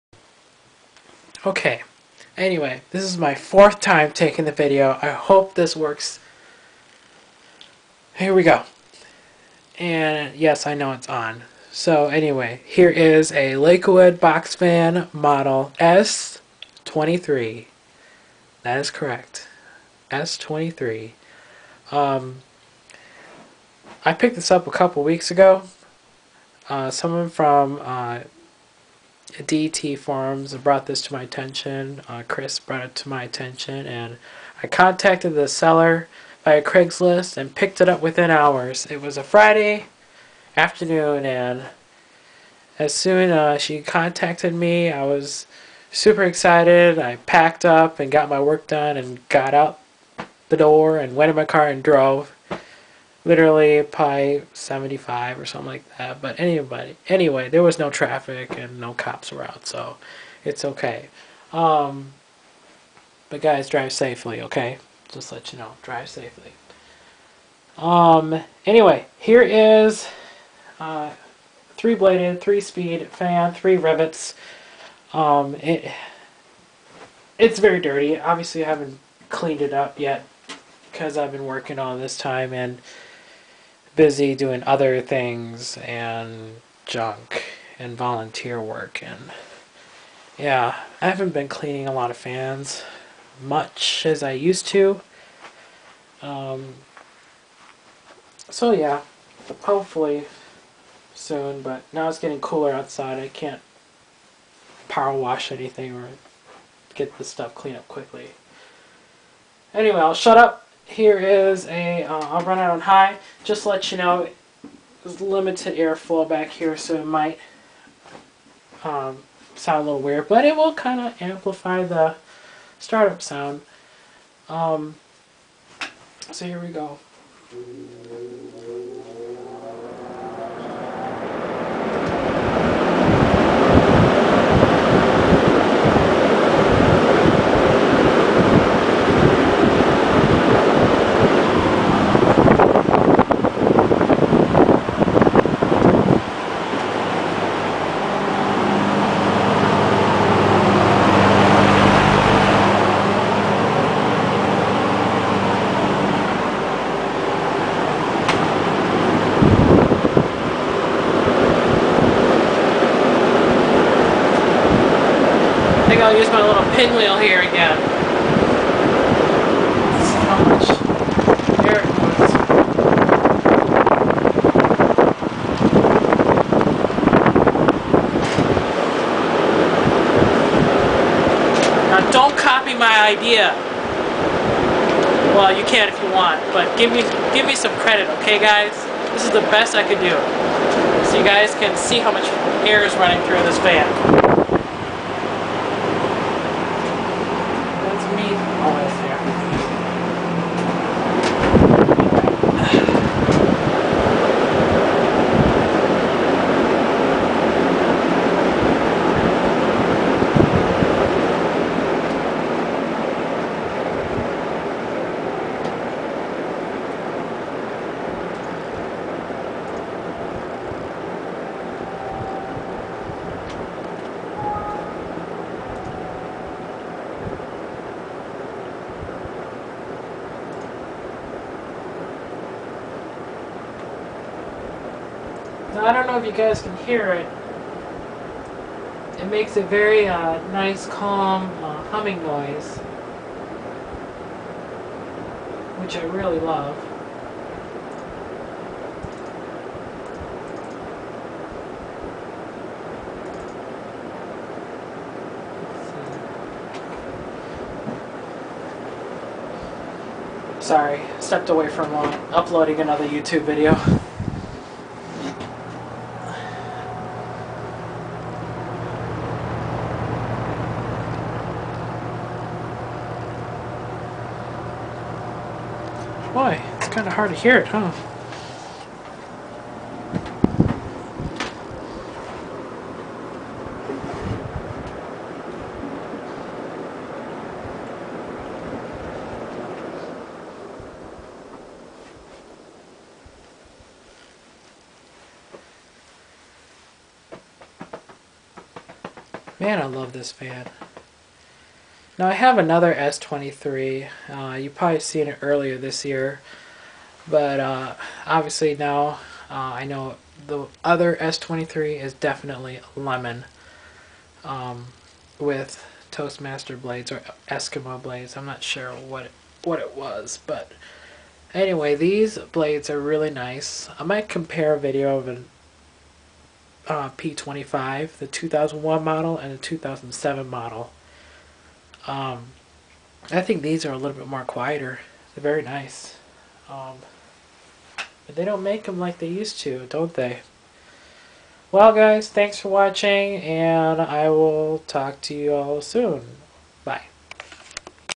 Lakewood S-23
Lakewood Box Fan Model S-23. Yes it will need some cleaning up. but here is a video since i've havant uploaded a few in a while.